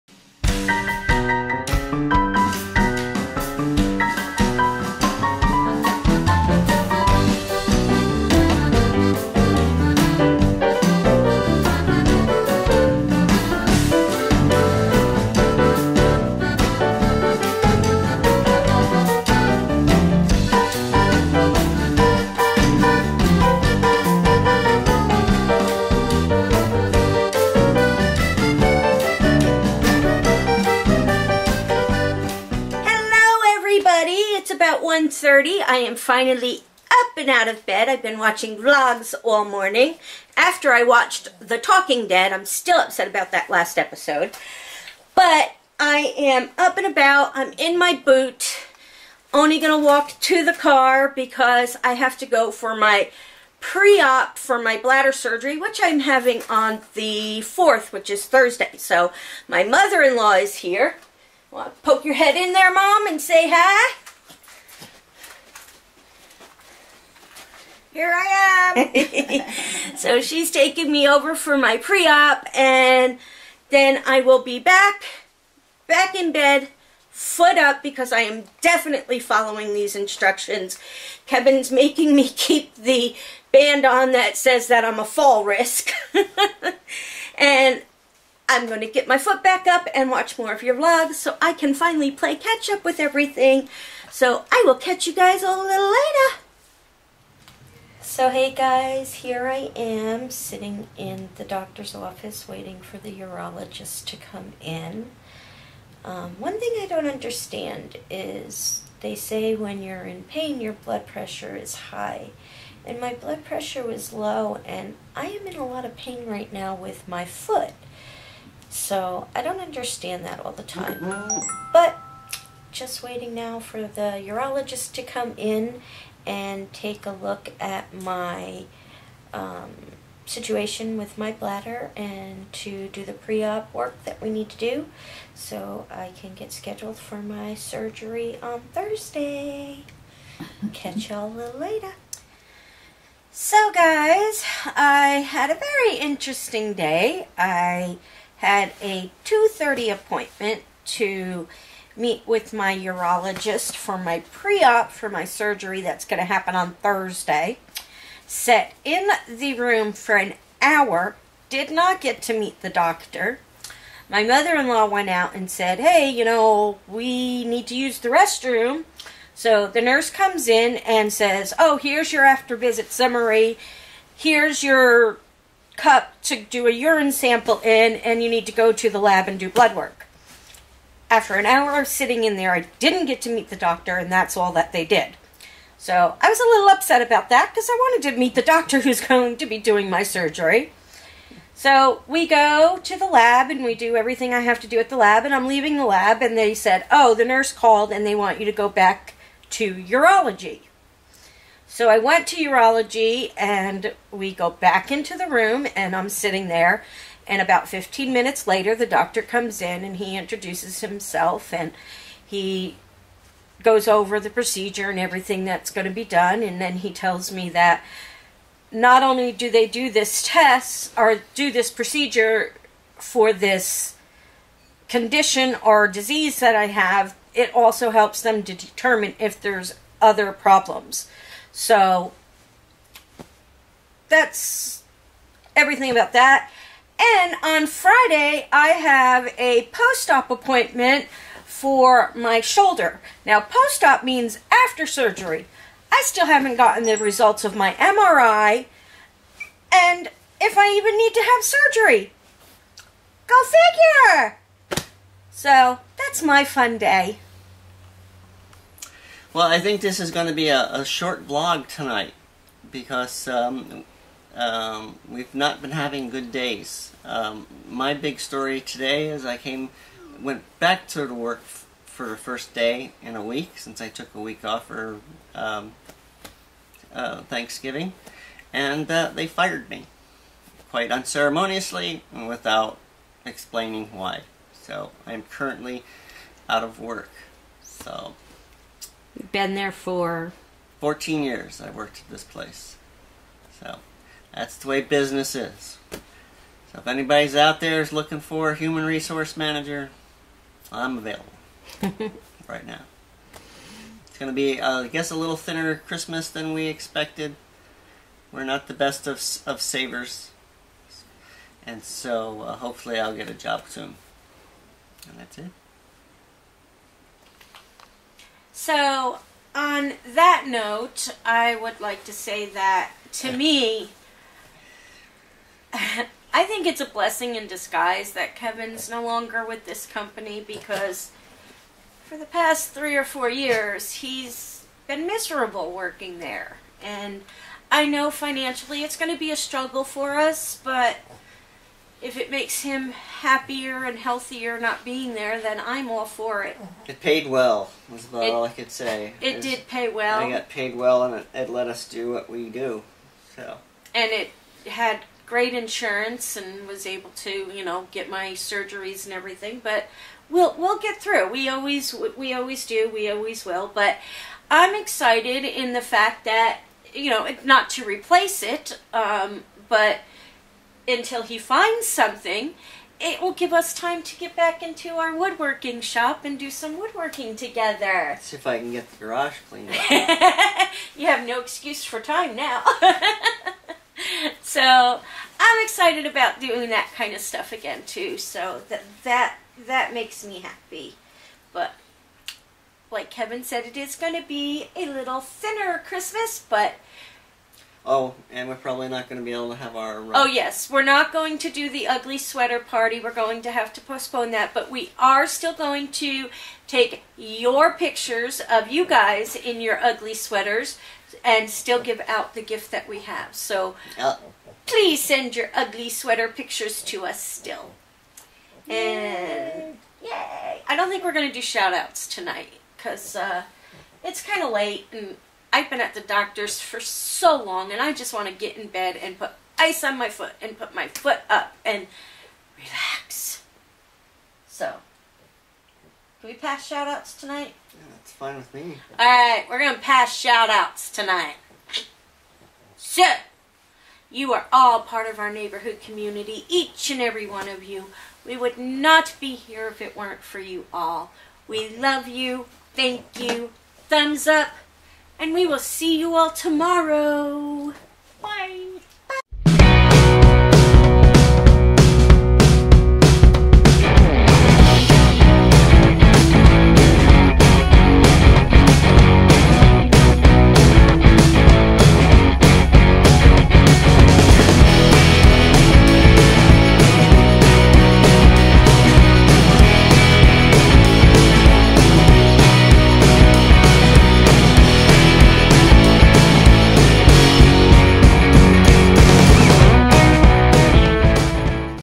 BAD DAY AT THE NEIGHBORS
Another long day at the Drs. for Val.  Some real bad news for Kevin.